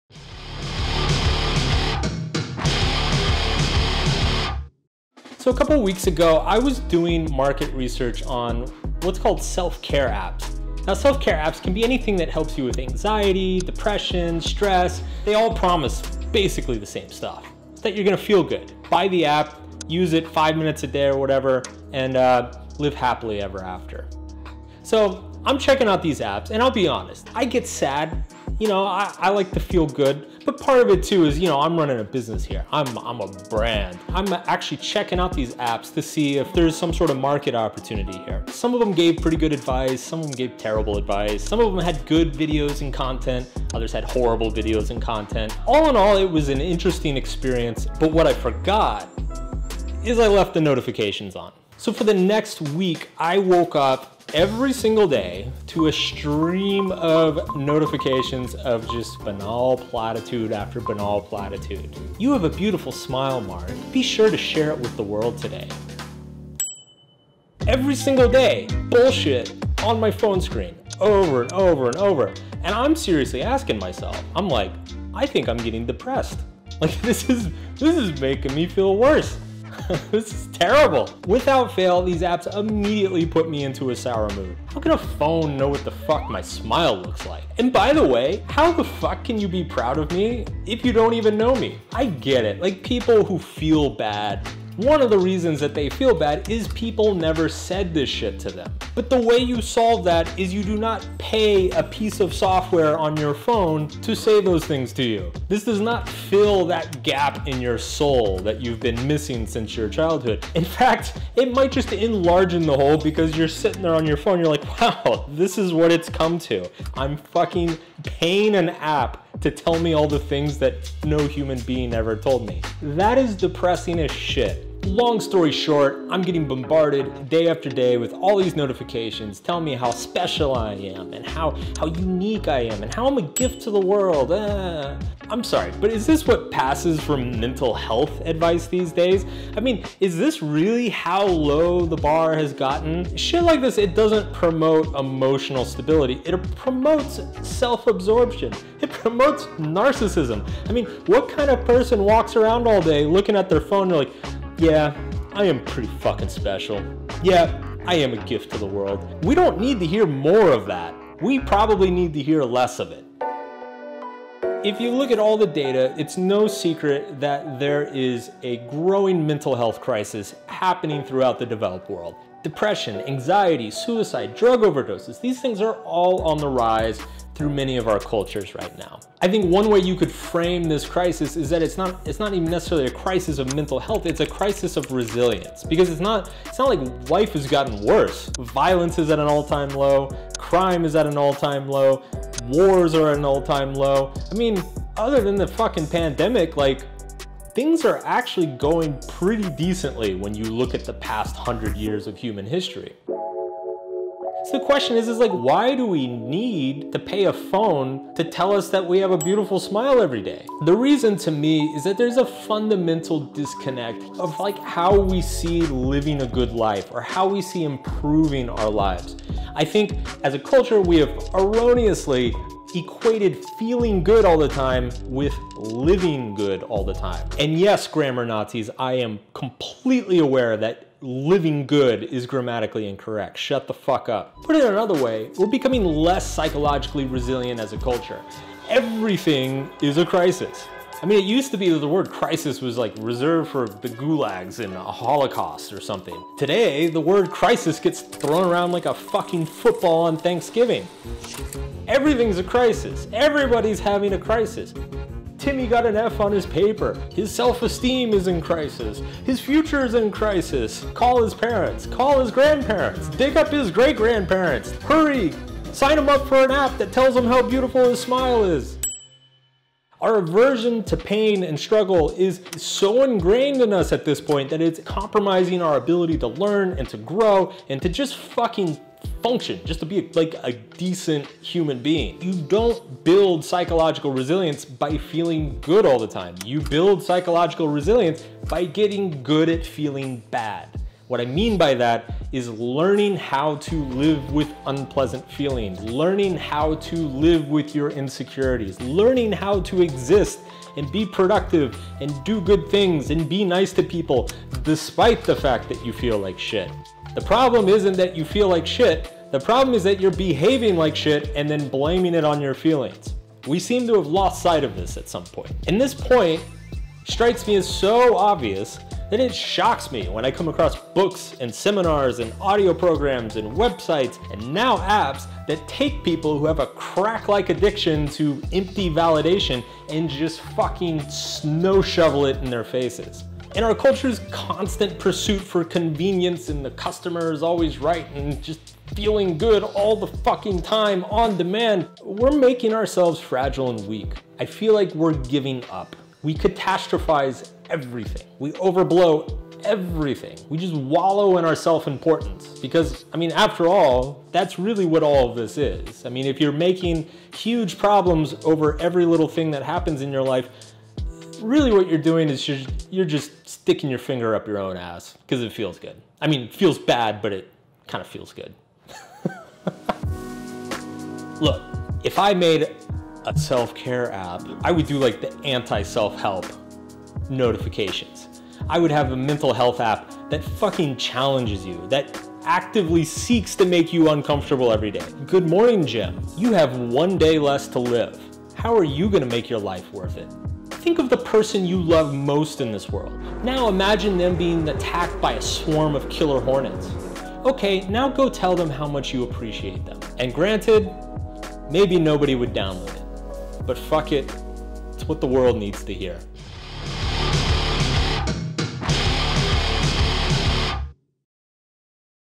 How to Get Good at Feeling Bad
Developing resilience doesn’t mean feeling good all the time. You need to embrace pain, it's there to help us.

If you’ve downloaded any of the mental-health or “self-care” apps available today, you may have noticed your phone turn into a digital ass-kisser that feeds you a bunch of narcissistic fluff each morning.

“You have a beautiful smile, share it with the world.”
“Achieve whatever you want to achieve today, just believe in yourself.”
“Everyday is an opportunity and today is yours. I’m proud of you!”

I’m sorry, but if affirmations and positive statements telling me how special I am is what passes for “mental health” advice these days, then we’re just pouring kerosene on a goddamn dumpster fire. Because this doesn’t promote emotional stability; it promotes self-absorption.

If you are not already a member of my premium membership, get access to my courses and exclusive writing

I share other types of content to make you a less awful human in these places: